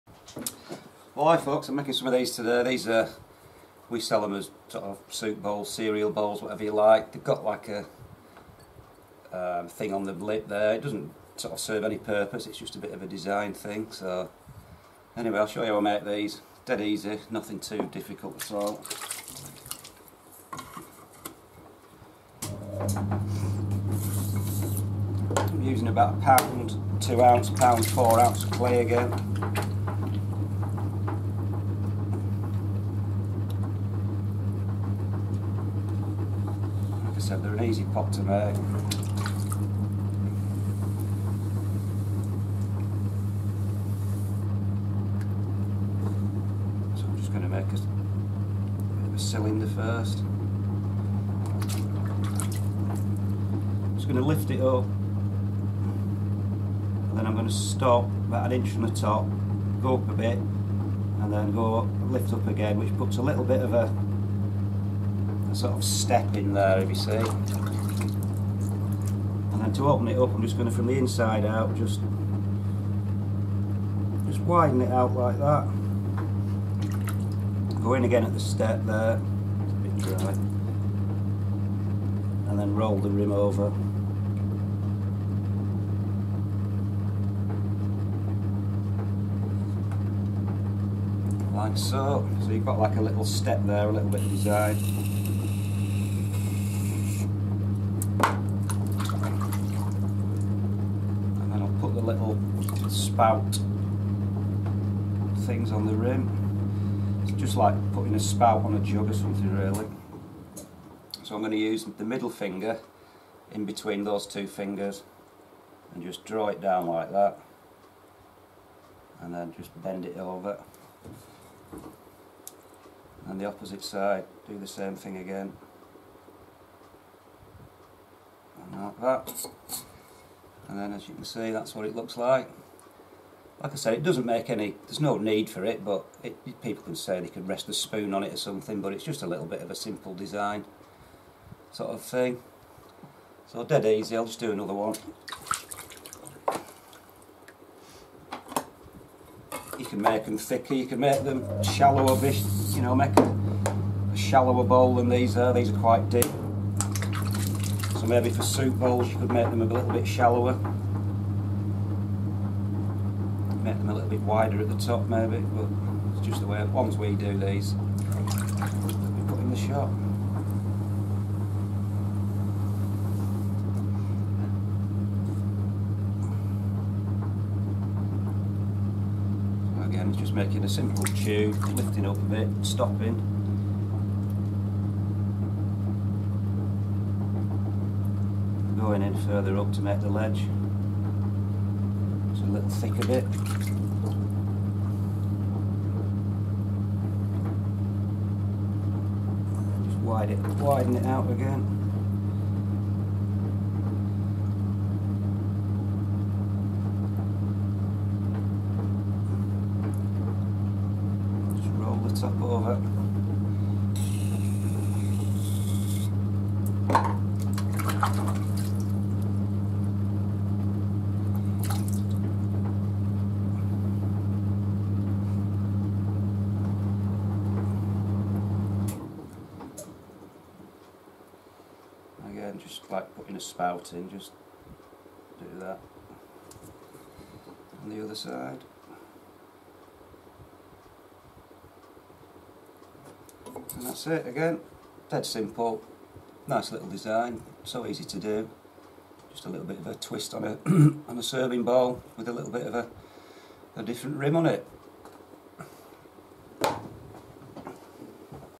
Throwing / Making some simple Pottery serving Bowls on the Wheel.
Watch some pottery instead of all this sidemen diss rubbish going on ;)